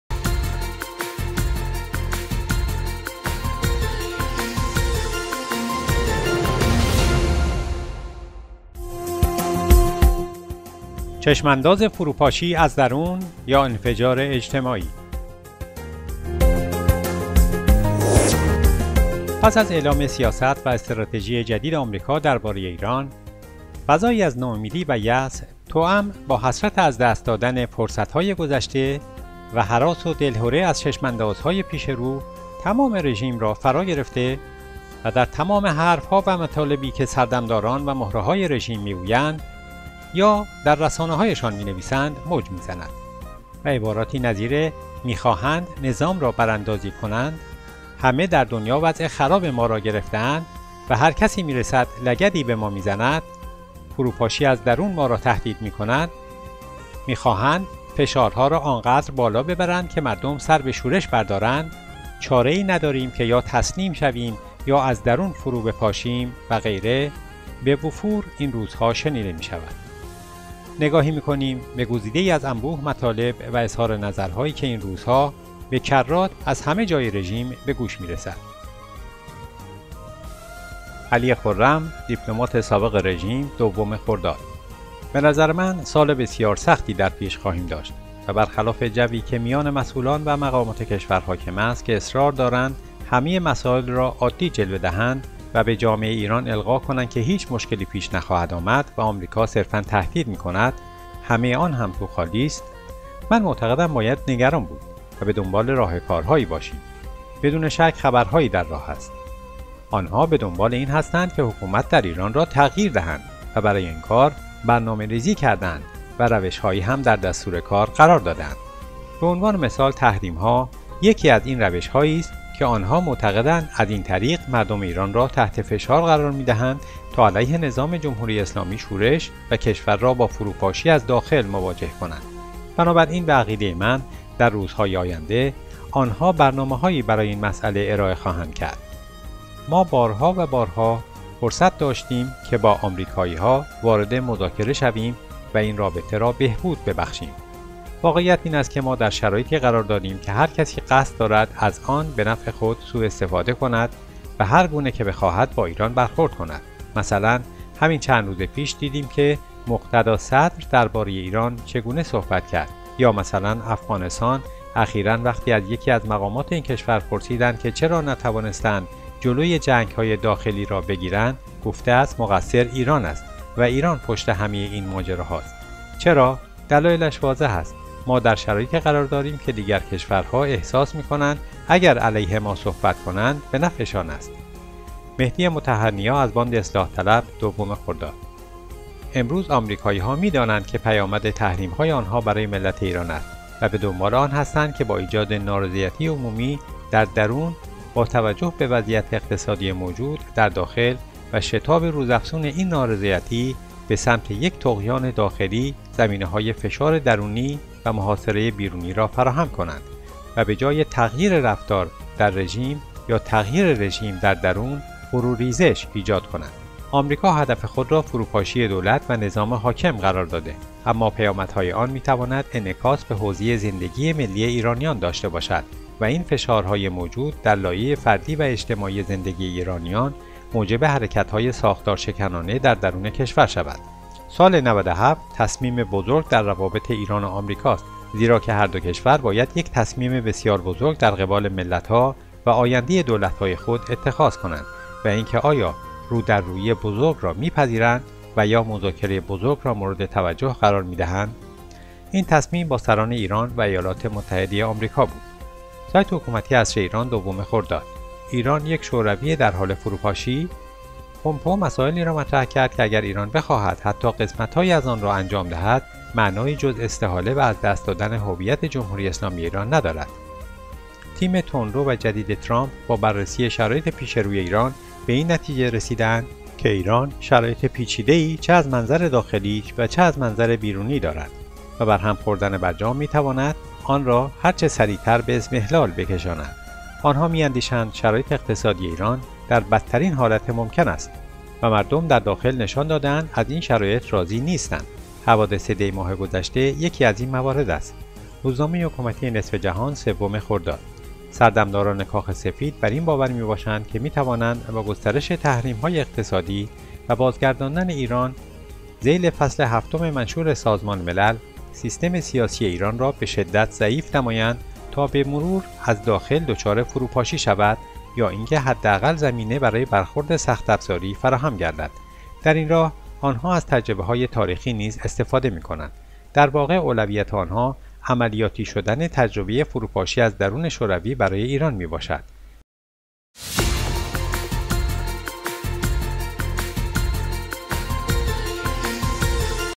گزارش خبری -چشم انداز فروپاشی از درون یا انفجار اجتماعی و براندازی؟
چشم‌انداز؛ فروپاشی از درون، یا انفجار اجتماعی؟
پس از اعلام سیاست و استراتژی جدید آمریکا دربارة‌ ایران فضایی از ناامیدی و یأس توآم با حسرت از دست دادن فرصتهای گذشته و هراس و دلهره از چشم‌اندازهای پیش رو، تمام رژیم را فراگرفته و در تمام حرفها و مطالبی که سردمداران و مهره‌های رژیم می‌گویند یا در رسانه‌هایشان می‌نویسند موج می‌زند و عباراتی نظیر می‌‌خواهند نظام را براندازی کنند، همه در دنیا وضع خراب ما را گرفته‌اند و هر کس می‌رسد لگدی به ما می‌زند؛ فروپاشی از درون ما را تهدید می‌کنند، می‌خواهند فشارها را آن قدر بالا ببرند که مردم سر به شورش بردارند؛ چاره‌یی نداریم که یا تسلیم شویم یا از درون فرو بپاشیم و غیره، به وفور این روزها شنیده می‌شود. اکنون نگاهی می‌کنیم به گزیده‌یی از انبوه مطالب و اظهارنظرهایی که این روزها به کرات از همه جای رژیم به گوش می‌رسد:

علی خرم دیپلماتی سابق رژیم ۲خرداد
به نظر من سال بسیار سختی در پیش خواهیم داشت و برخلاف جوی که میان مسئولان و مقامات کشور حاکم است که اصرار دارند همه مسائل را عادی جلوه دهند به جامعه این را القا کنند که هیچ مشکلی پیش نخواهد آمد و آمریکا صرفا تهدید می‌کند همه آن هم توخالی است؛ من معتقدم باید نگران بود و به دنبال راهکار‌هایی باشیم. بدون شک خبر‌هایی در راه است. آن‌ها به دنبال این هستند که حکومت در ایران را تغییر دهند و برای این کار برنامه ریزی کرده‌اند و روش‌هایی هم در دستور کار قرار داده‌اند
. به عنوان مثال تحریم‌ها یکی از این روش‌هایی است که آن‌ها معتقدند از این طریق مردم ایران را تحت فشار قرار می‌دهند تا علیه نظام جمهوری اسلامی شورش و کشور را با فروپاشی از داخل مواجه کنند؛ بنابراین به عقیده من در روز‌های آینده آن‌ها برنامه‌هایی برای این مسئله ارائه خواهند کرد.»
ما بار‌ها وبار‌ها فرصت داشتیم که با آمریکایی‌ها وارد مذاکره شویم و این رابطه را بهبود ببخشیم
واقعیت این است که ما در شرایطی قرار داریم که هرکسی قصد دارد از آن به نفع خود سواستفاده کند و هرگونه که بخواهد با ایران برخورد می‌کند. مثلا همین چند روز پیش دیدیم که مقتدی صدر درباره ایران چگونه صحبت کرد یا مثلا افغانستان اخیرا وقتی از یکی از مقامات این کشور پرسیدند که «چرا نتوانسته‌اند جلوی جنگ‌های داخلی را بگیرند؟ گفته است مقصر ایران است و ایران پشت همه این ماجرا‌ها است.» چرا؟! دلایلش واضح است. ما در شرایطی قرار داریم که دیگر کشور‌ها احساس می‌کنند اگر علیه ما صحبت کنند، به نفعشان است.»

مهدی مطهر نیا از باند اصلاح طلب ۲ خرداد
امروز آمریکایی‌ها می‌دانند که پیامد تحریم‌های آنها برای ملت ایران است و به دنبال آن هستند که با ایجاد نارضایتی عمومی در درون با توجه به وضعیت اقتصادی موجود در داخل و شتاب روز افزون این نارضایتی به سمت یک طغیان داخلی زمینه‌های فشار درونی و محاصره بیرونی را فراهم کنند و به جای تغییر رفتار در رژیم یا تغییر رژیم در درون فرو ریزش ایجاد کنند
آمریکا هدف خود را فروپاشی دولت و نظام حاکم قرار داده‌ اما پیامد‌های آن می‌تواند انعکاسی به حوزه زندگی ملی ایرانیان داشته باشد و این فشار های موجود در لایه فردی و اجتماعی زندگی ایرانیان موجب حرکت‌های ساختارشکنانه در درون کشور شود.»
سال 97 سال  تصمیم بزرگ در روابط ایران و آمریکا است زیرا که هر دو کشور باید یک تصمیم بسیار بزرگ در قبال ملت‌ها و آینده دولت‌های خود اتخاذ کنند و اینکه آیا رودرویی بزرگ را می‌پذیرند و یا مذاکره بزرگ را مورد توجه قرار می دهند. این تصمیم با سران ایران و ایالات متحده آمریکا بود
سایت حکومتی عصر ایران ۲ خرداد
ایران یک شوروی در حال فروپاشی؟
پمپئو مسائلی را مطرح کرد که اگر ایران بخواهد حتی قسمت هایی از آن را انجام دهد معنایی جز استحاله و از دست دادن هویت جمهوری اسلامی ایران ندارد.
تیم تندرو و جدید ترامپ با بررسی شرایط پیش روی ایران به این نتیجه رسیده اند که ایران شرایط پیچیده ای چه از منظر داخلی و چه از منظر بیرونی دارد و برهم خوردن برجام می تواند آنرا هر سریعتر به اضمحلال بکشاند. آنها می اندیشند شرایط اقتصادی ایران در بدترین حالت ممکن است و مردم در داخل نشان داده اند از این شرایط راضی نیستند.
حوادث دی ماه گذشته یکی از این موارد است.
روزنامه حکومتی نصف جهان ۳ خرداد
سردمداران کاخ سفید بر این باور می باشند که می توانند با گسترش تحریم های اقتصادی و بازگرداندن ایران ذیل فصل هفتم منشور سازمان ملل، سیستم سیاسی ایران را به شدت ضعیف نمایند تا به مرور از داخل دچار فروپاشی شود یا اینکه حداقل زمینه برای برخورد سخت افزاری فراهم گردد. در این راه آنها از تجربه های تاریخی نیز استفاده می کنند. در واقع اولویت آنها عملیاتی شدن تجربه فروپاشی از درون شوروی برای ایران می باشد.